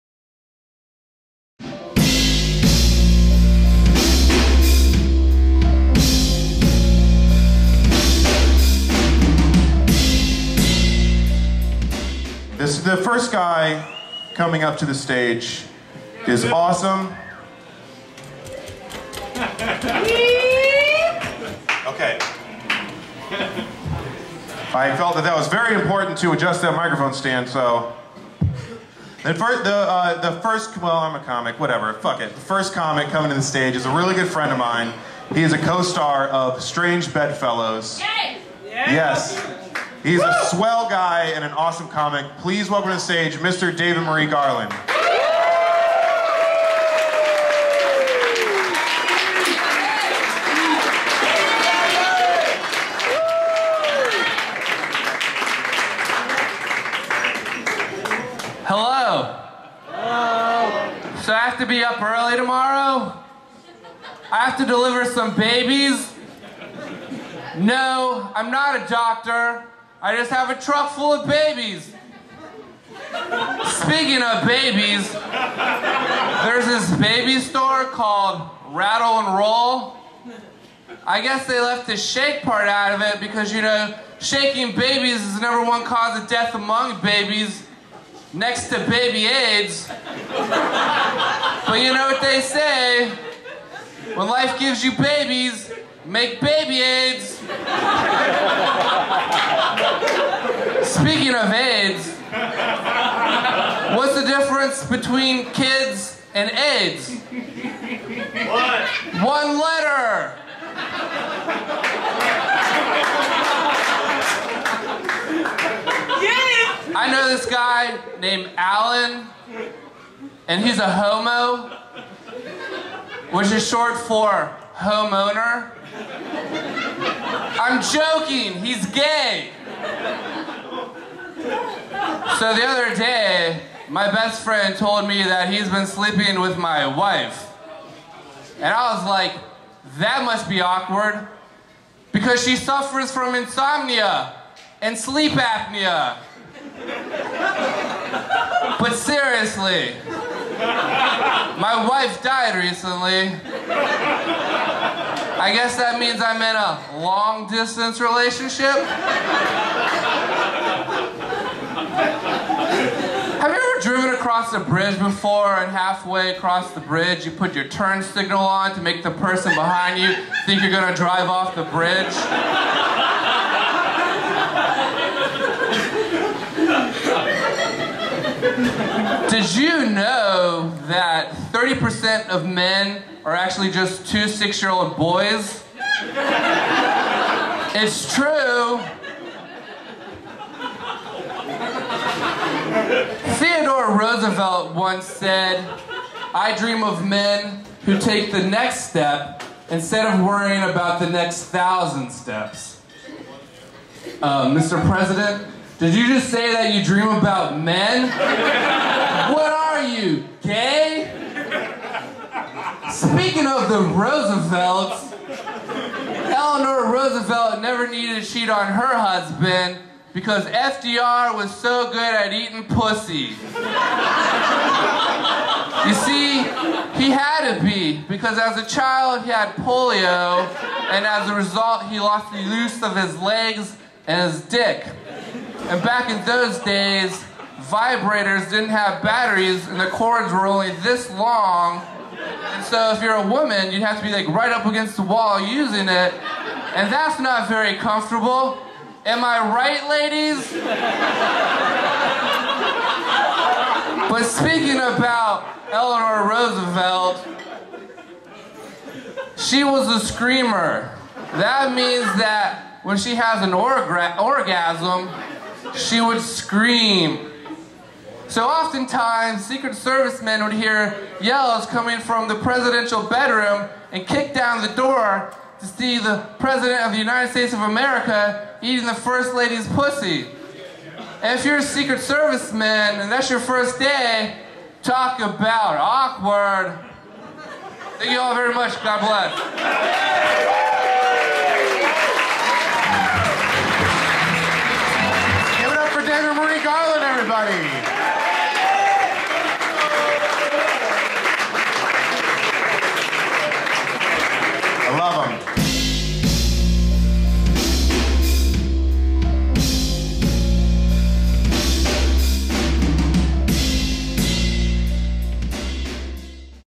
David Marie Garland - 2012 March 15 - Live at Strange Matter - Richmond, Virginia
Bill Metzger introduced David Marie Garland at Strange Matter in Richmond, Virginia.

David Marie Garland was one of the opening acts for Kyle Kinane, whose set can be watched